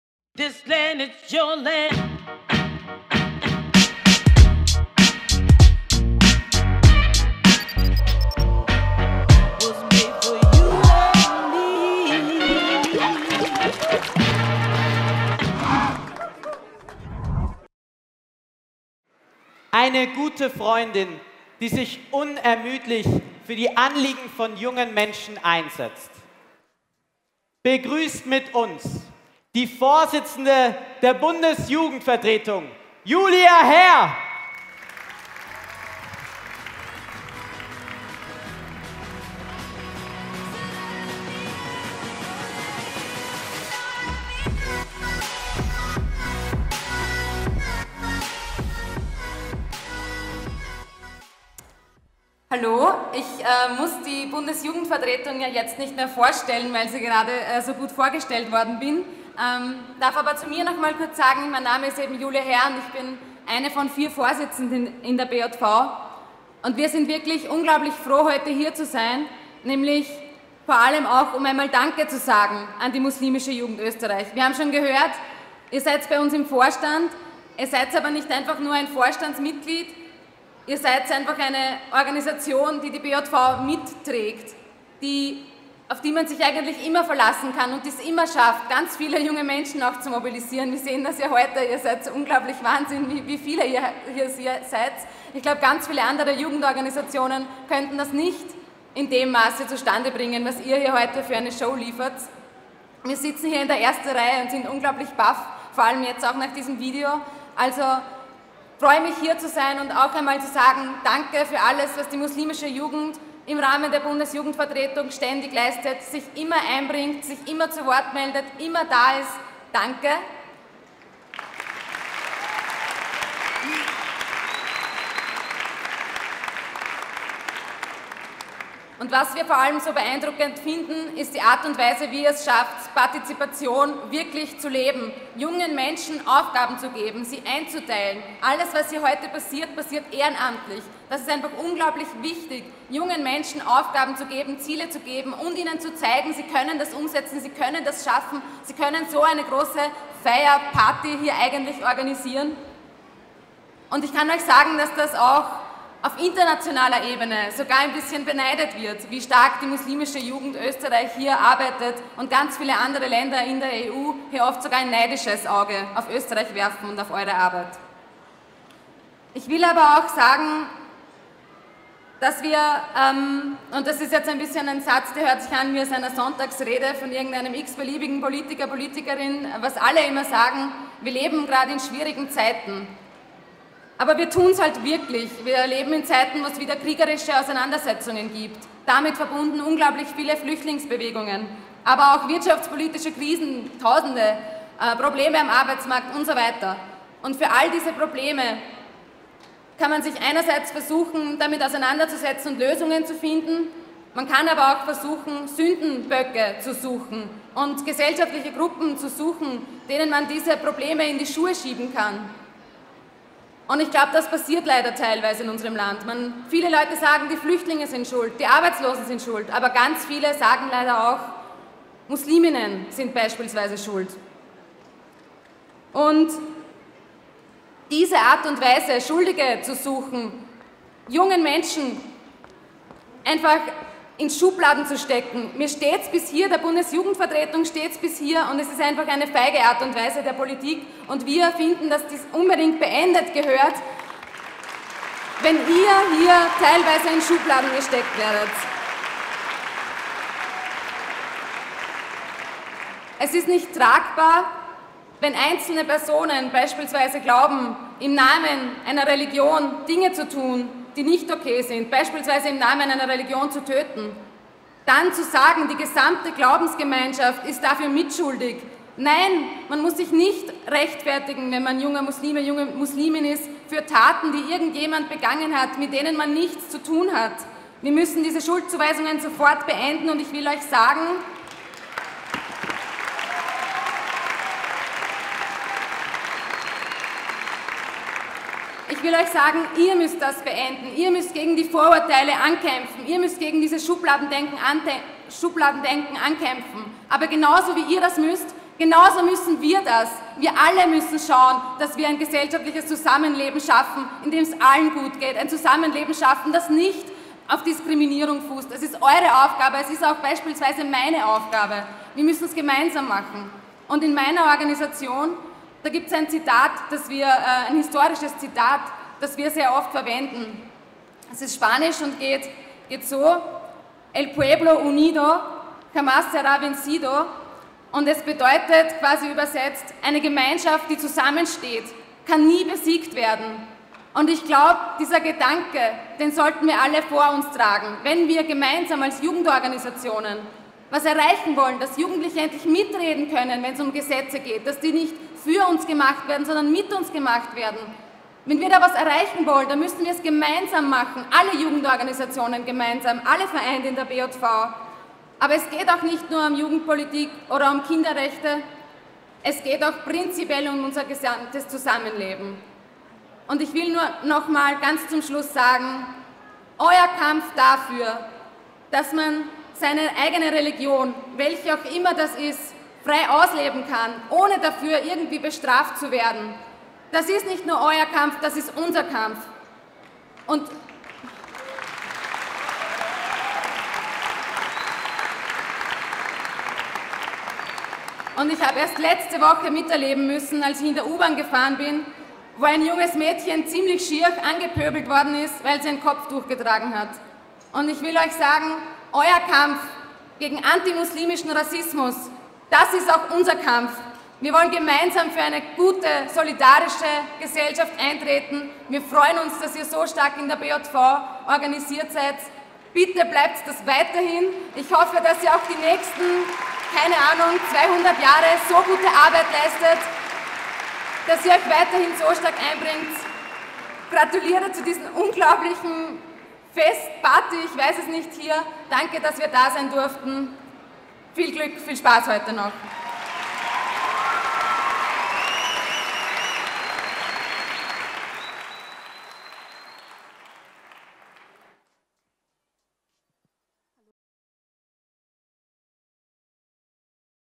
MJÖ 20-Jahresfeier-Rede: Julia Herr, BJV-Vorsitzende
So viele Jugendliche haben nochmal um ihre Rede gebeten. Julia Herr, Vorsitzende der Bundes Jugend Vertretung, hat sehr besondere und wichtige Worte an uns bei der 20 Jahresfeier gerichtet. Vielen Dank liebe Julia, dass du bei uns warst und danke für deine tolle Rede!